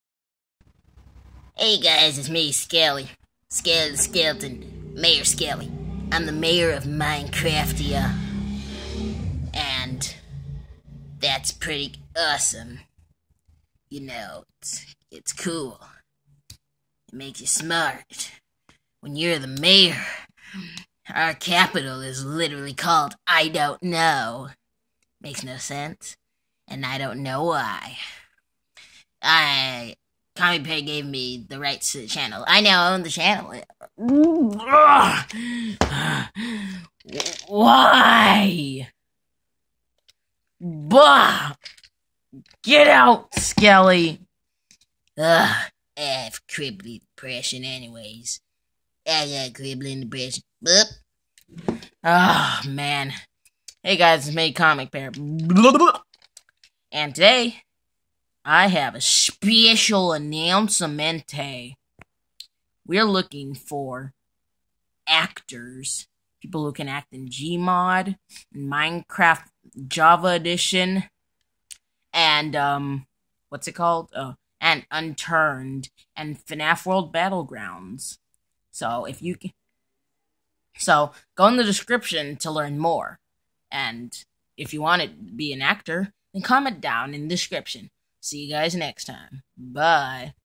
BE AN ACTOR! (Description)
You can be a Minecraft (Java), GMod, Unturned, FNaF World Battlegrounds, FNaF fan game, MonsterCrafter, or Robocraft actor. To join please answer these questions in the comments:
1. What type of actor can you be? (One or all)
2. If you want to be a Minecraft actor, do you have TechnicLauncher? (This is only required if you want to do modded roleplays)
3. What is your Steam or Discord? (We need this to communicate and add your voice to videos+if you're using Steam, send me a friend request)
4. If you are going to be a FNaF fan game actor, will you be able to download FNaF fan games that are multiplayer?